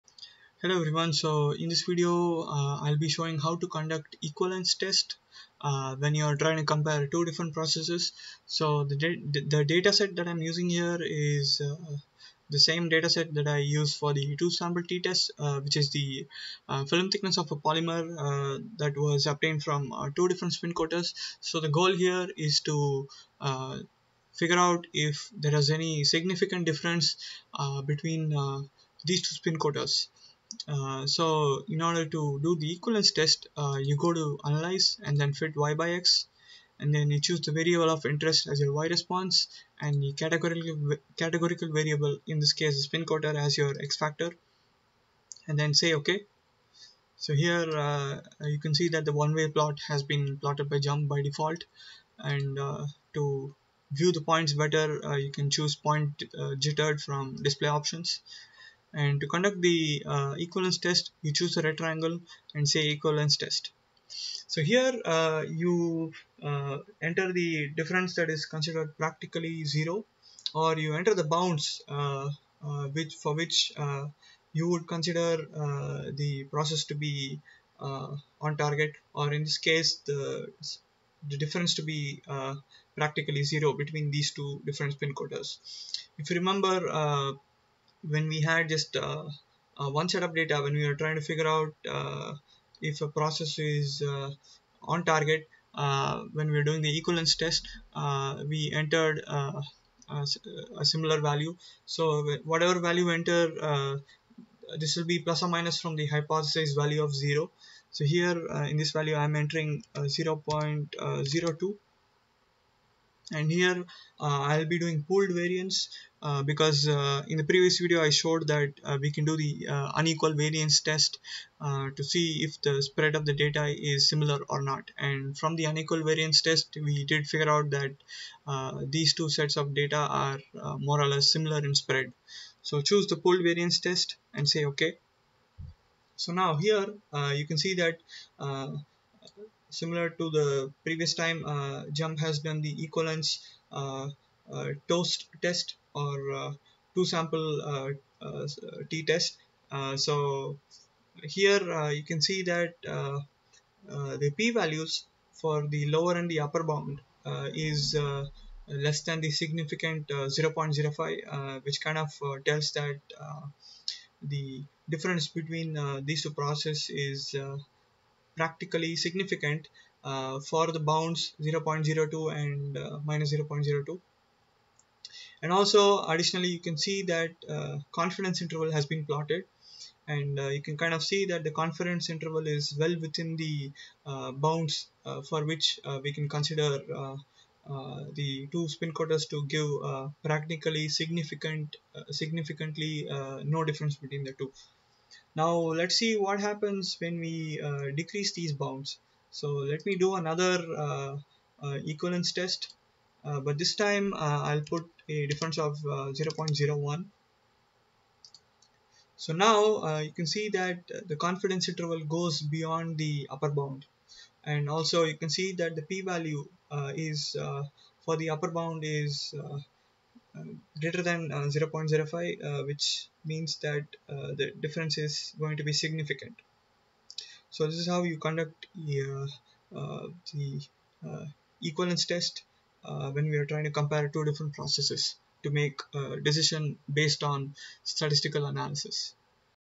Hypothesis testing - Equivalence test for comparing 2 means - JMP
This video shows how to conduct equivalence test to compare 2 process means in JMP.